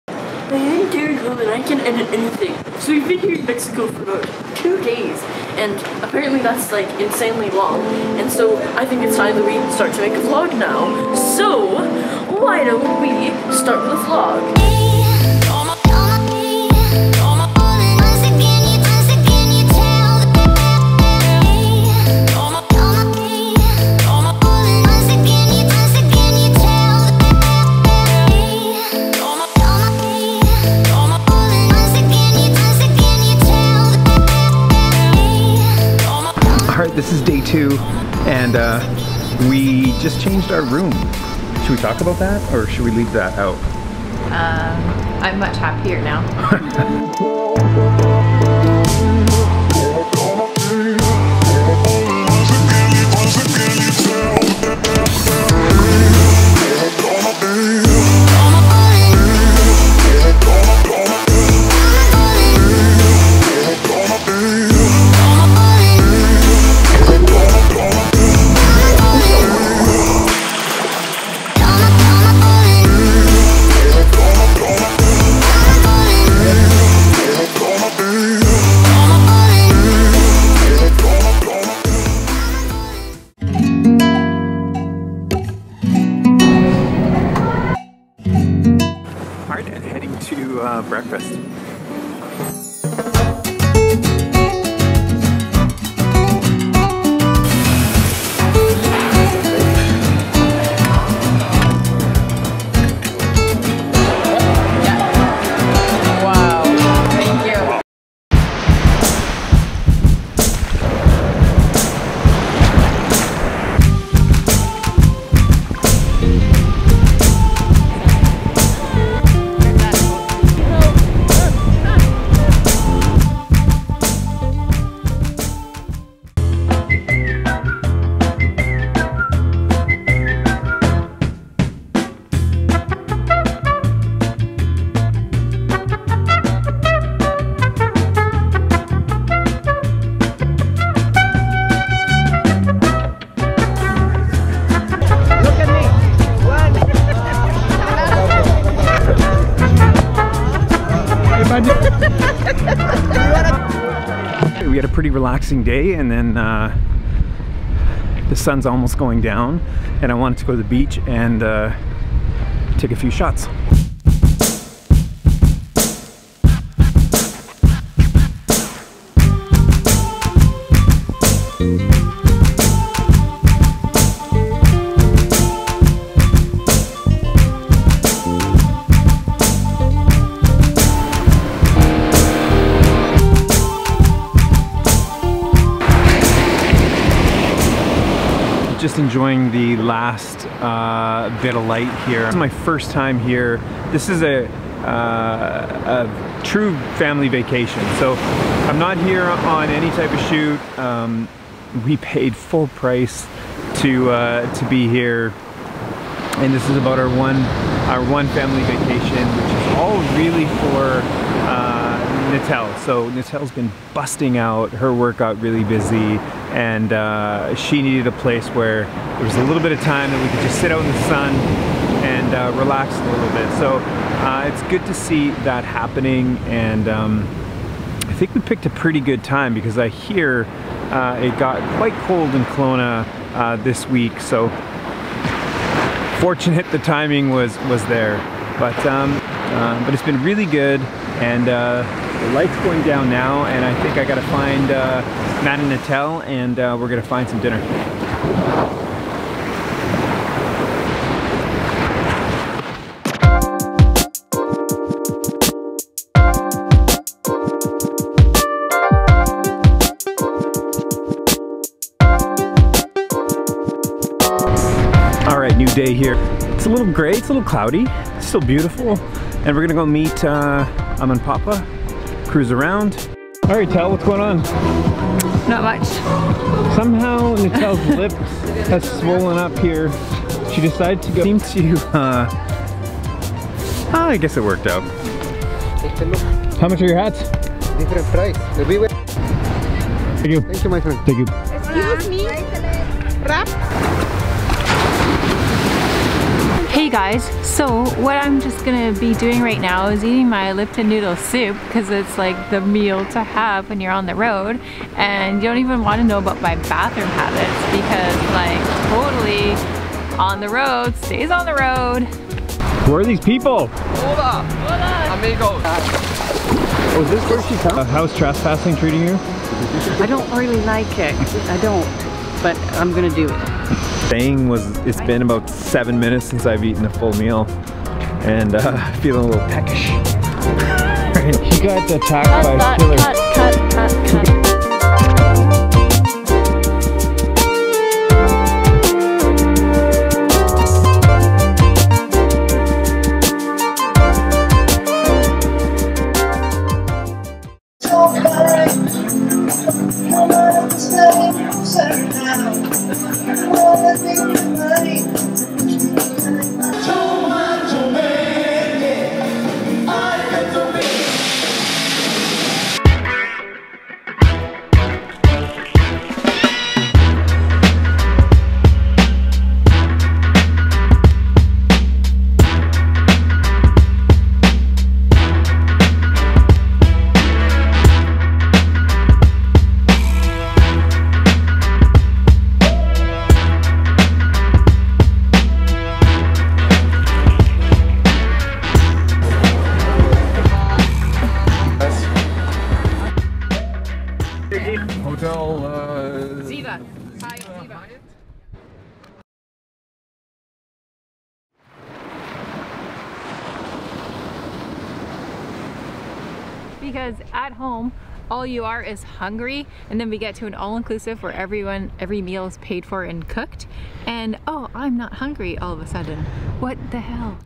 Los Cabos - Eating every 7 minutes. (I don't think my family likes me)
Vlog Season 2. Episode 2.
In Los Cabos for a week hanging with my FamJam at Hyatt Zevia.